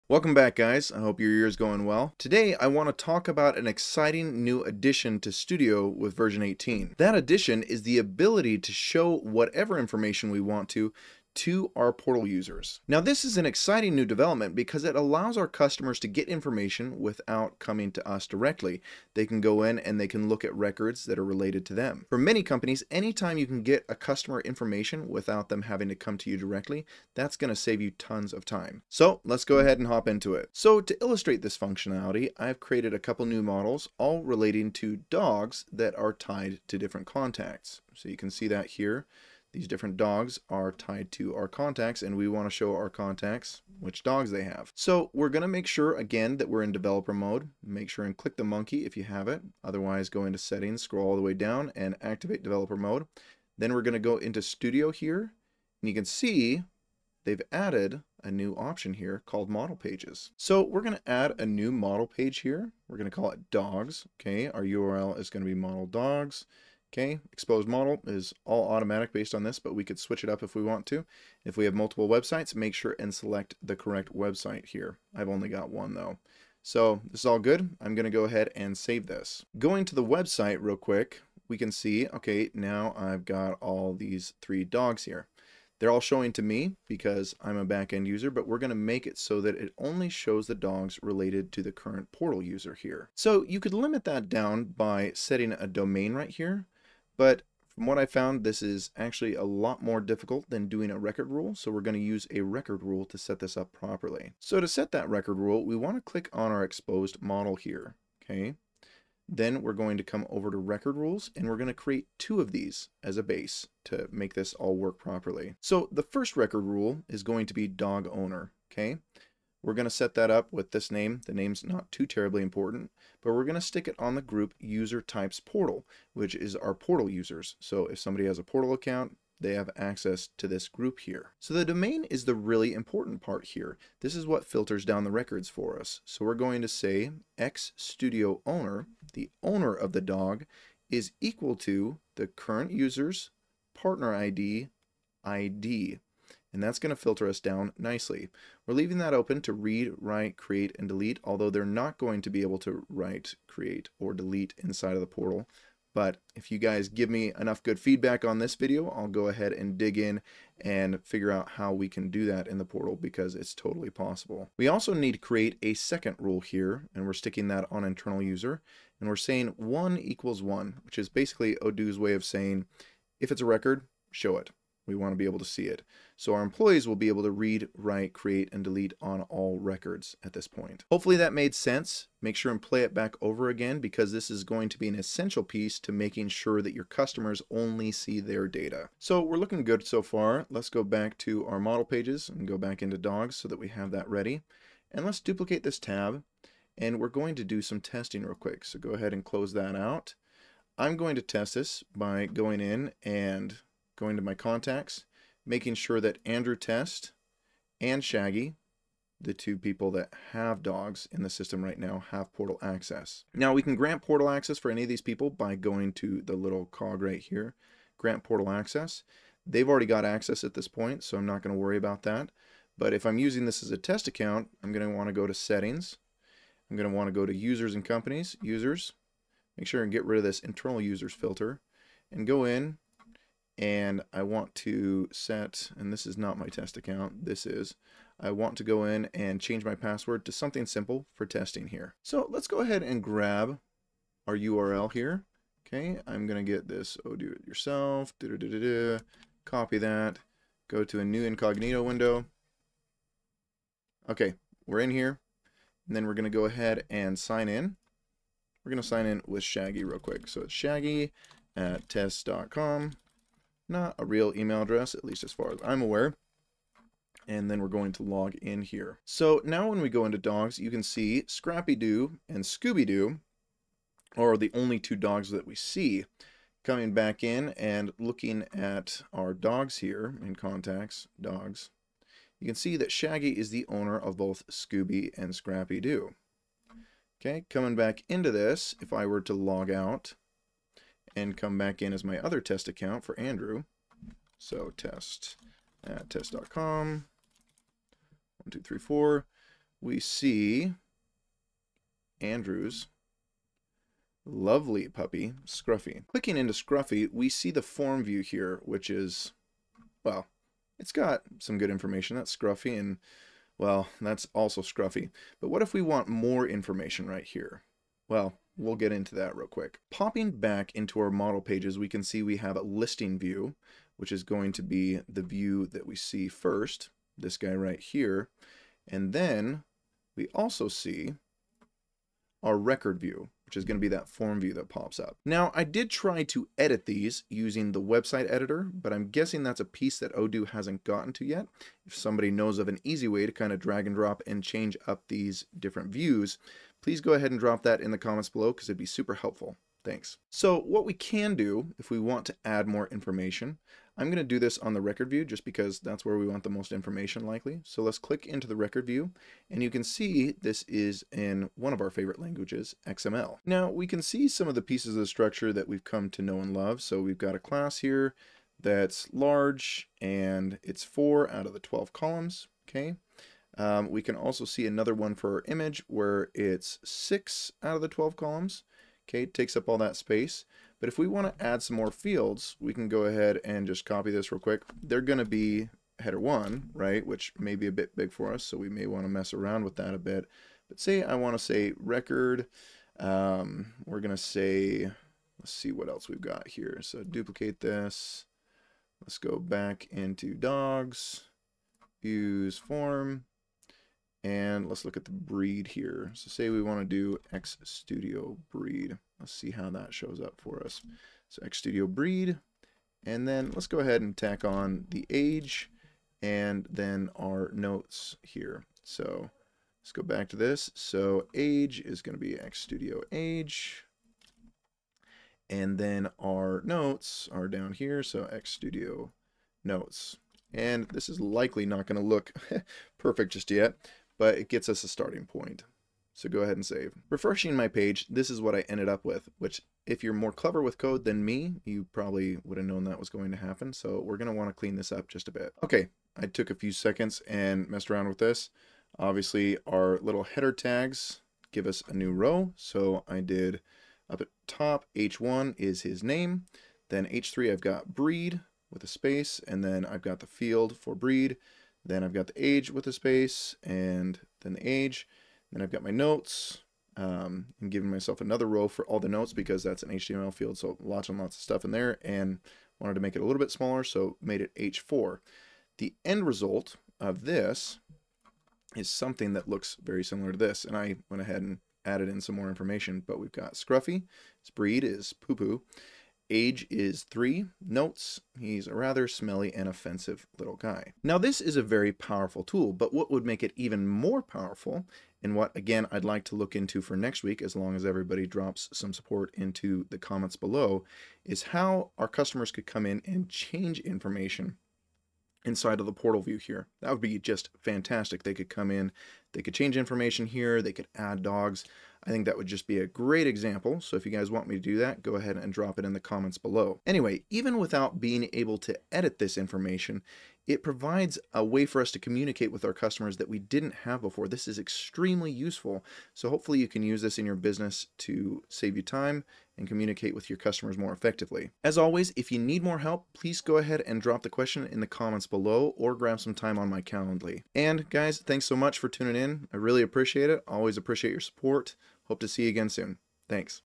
Creating Custom Views in the Customer Portal in Odoo
Have you ever wished you could show your customers more in the portal in Odoo?  You're not alone and now it's easier than ever! Let me show you how to easily add custom views to show your customers whatever information you want!